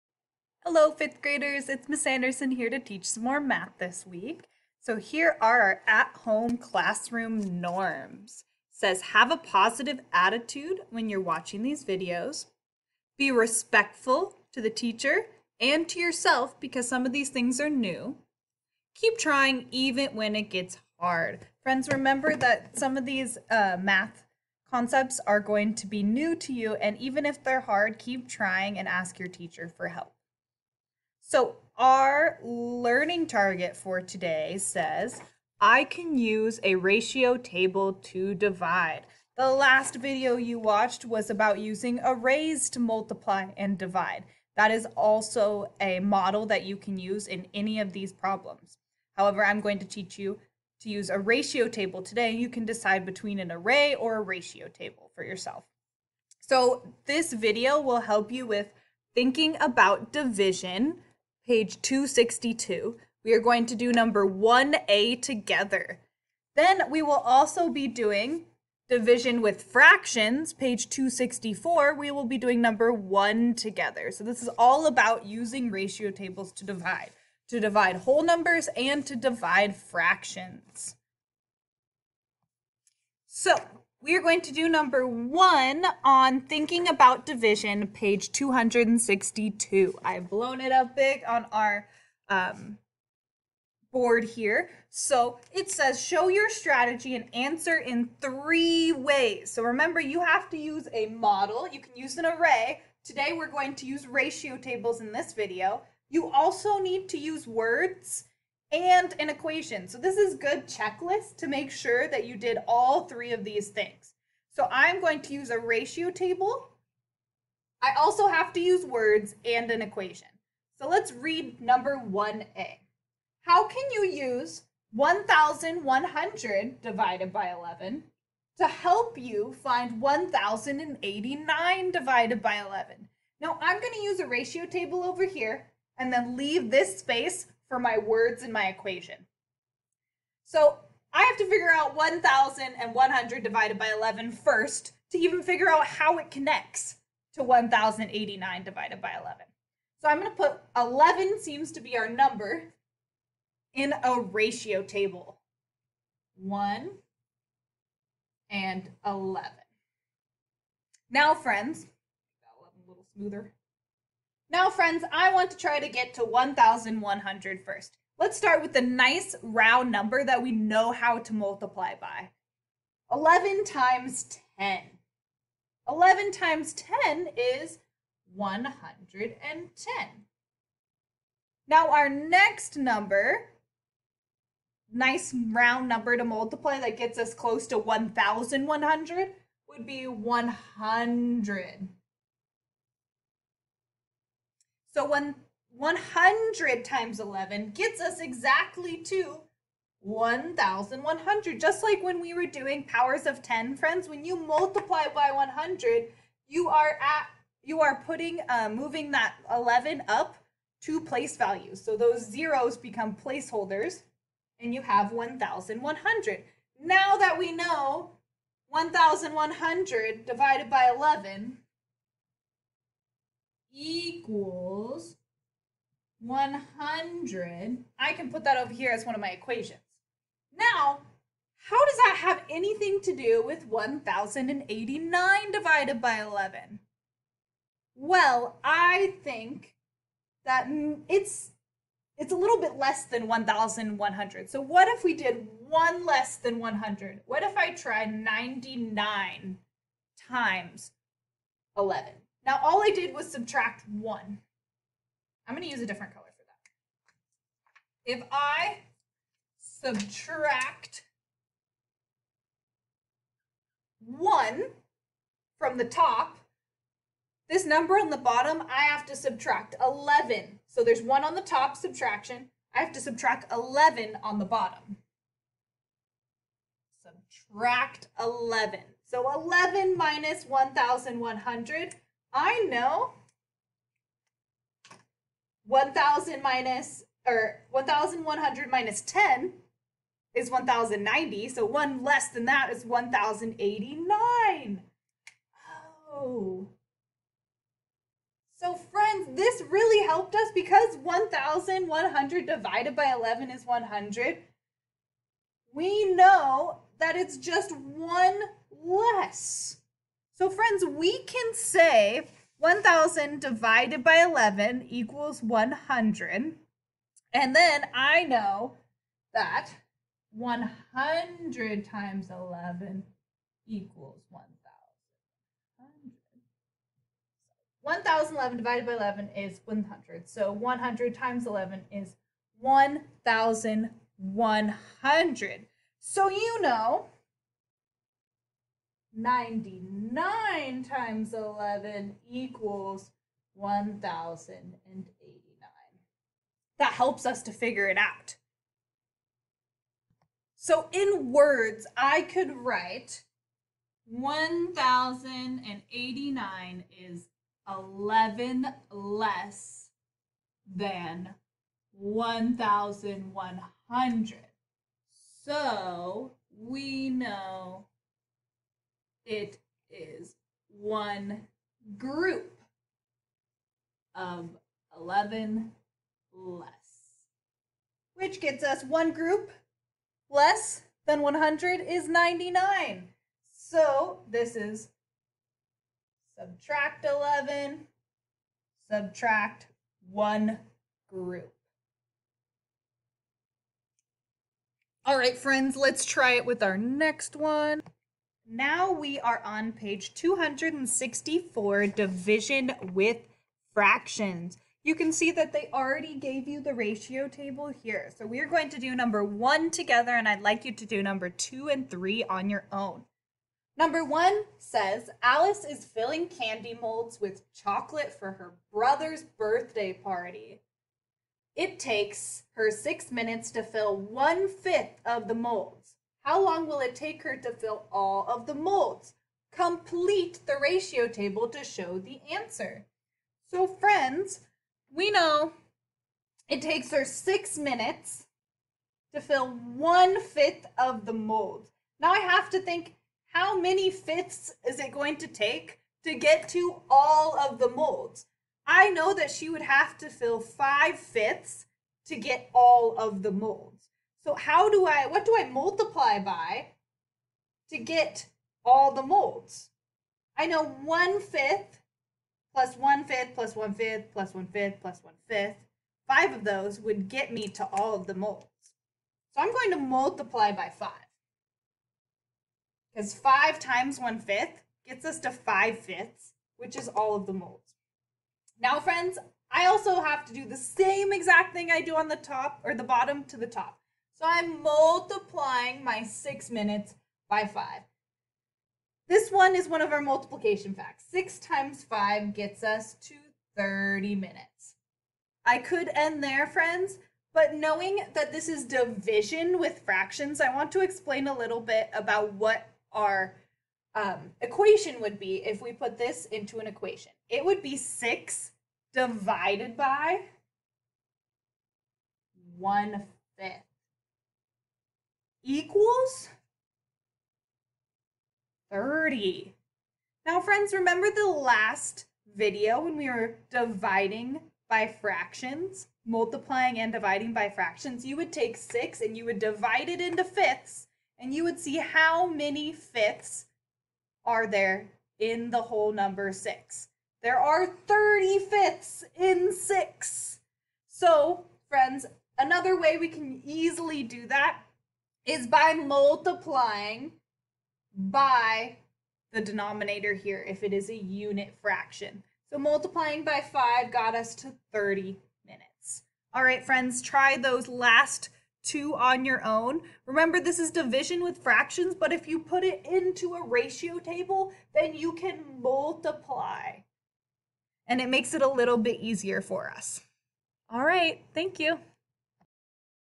5th Grade May 28th Math Lesson 9: Using Ratio Tables to Divide
Math Lesson 9: Using Ratio Tables to Divide